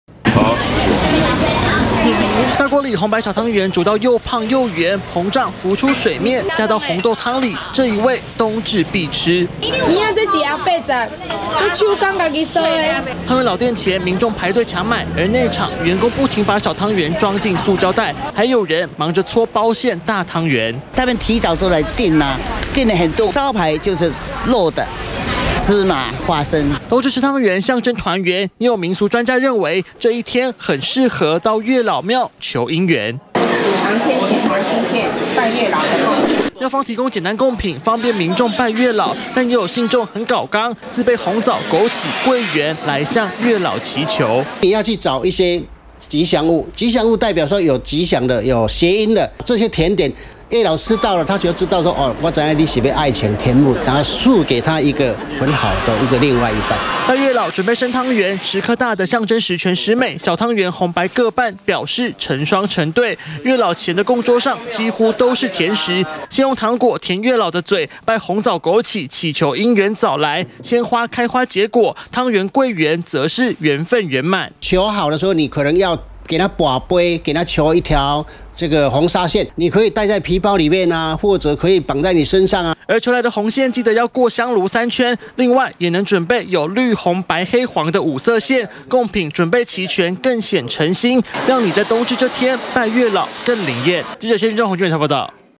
20191222中天新聞 冬至不只吃湯圓！ 拜月老祈求緣分「圓滿」
12/22就是冬至了，湯圓老店出現不少搶買人潮，冬至吃湯圓象徵團圓、圓滿，但其實這一天，也很適合拜月老，民俗專家說冬至準備湯圓等甜食，向月老祈求姻緣，也很靈驗。